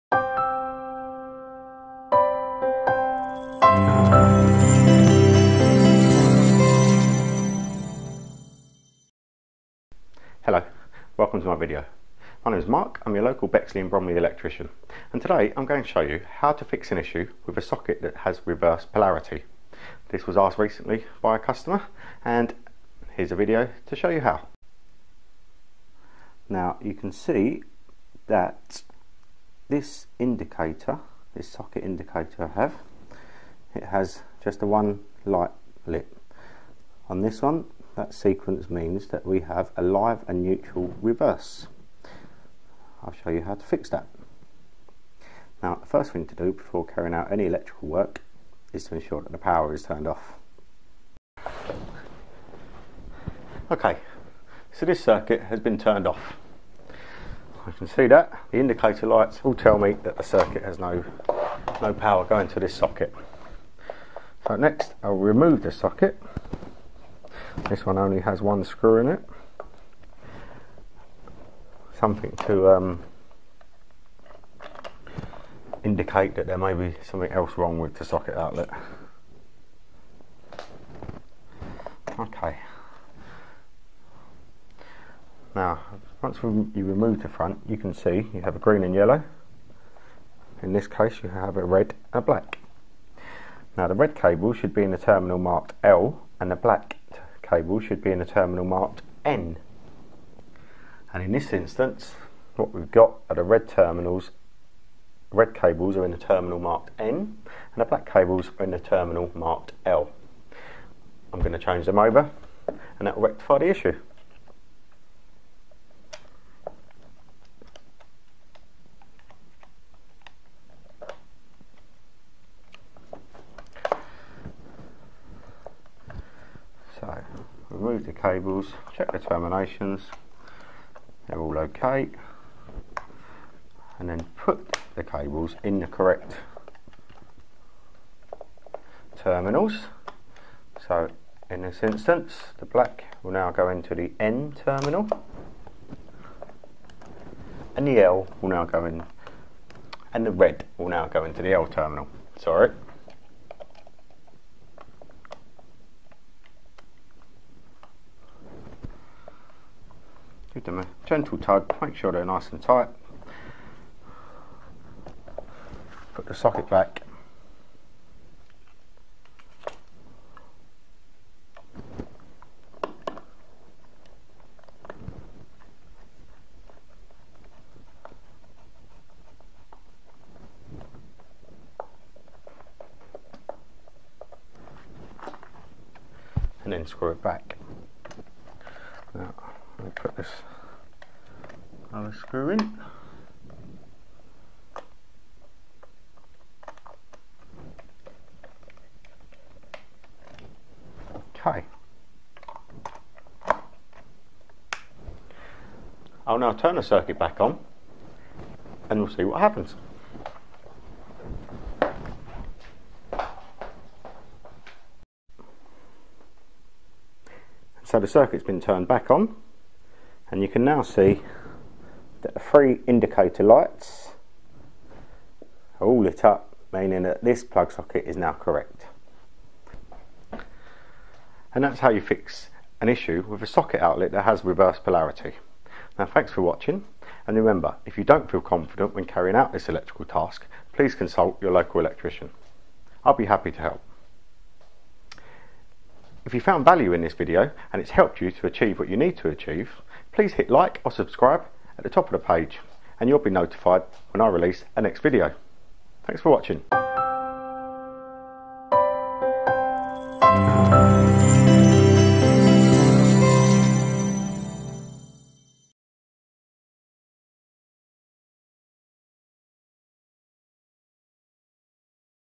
How to Fix a Socket that has Reverse Polarity.
Mark from M.T.R Electrics shows you how to rectify a reverse polarity issue with a plug socket in your home. Your local NICEIC Electricians in Bexley, Bromley, Dartford and Sevenoaks. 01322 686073.